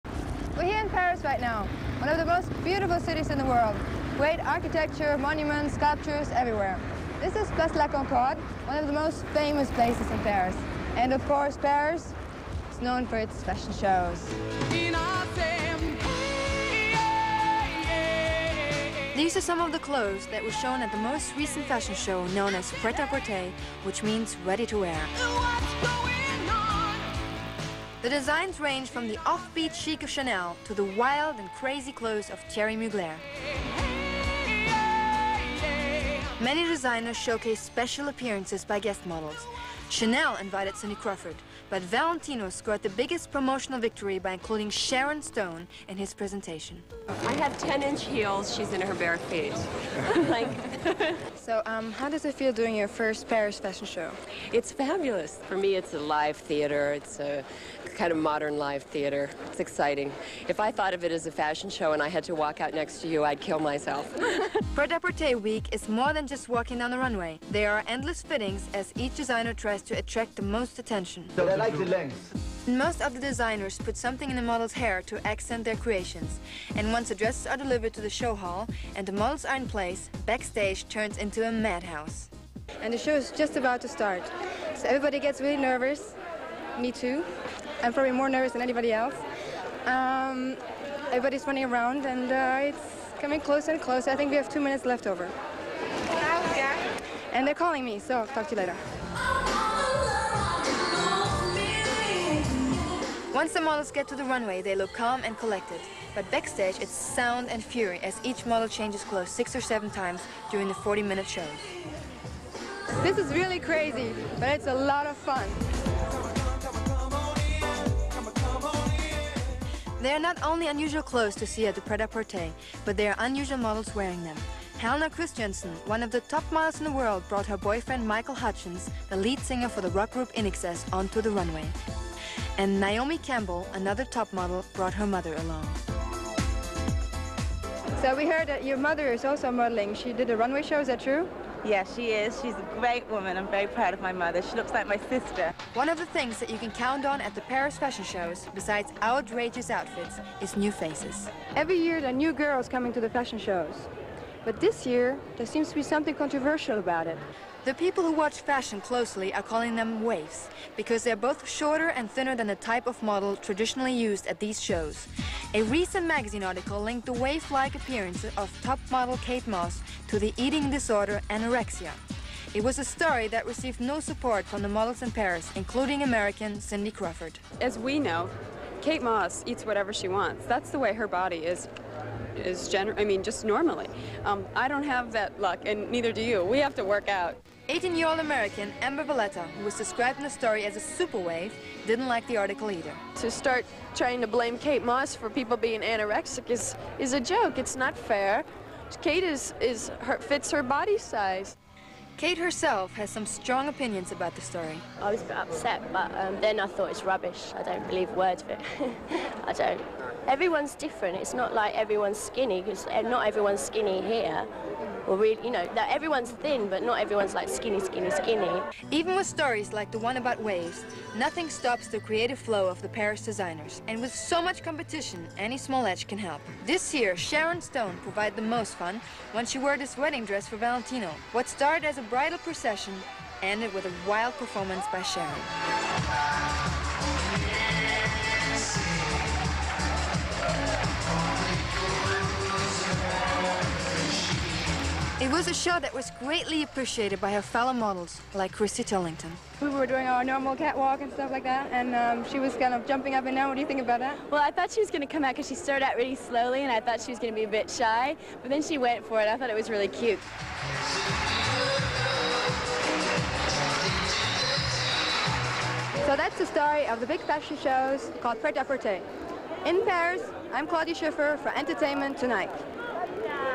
Claudia Schiffer - Paris Fashion Week Correspondent 1994
Claudia Schiffer goes to Paris backstage chaos and runway shows. Catching up with other supermodels.